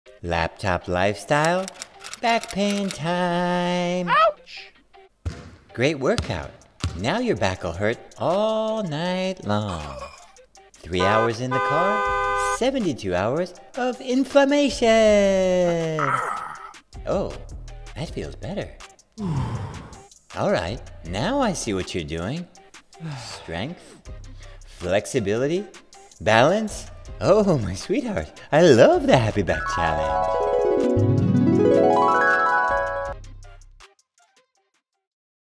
Back pain? Laptop lifestyle?
Back pain affects 80% of us at some point in life. The 21-Day Happy Back Challenge is designed to give you a correct exercise-based path forward with strength, stretching, and balance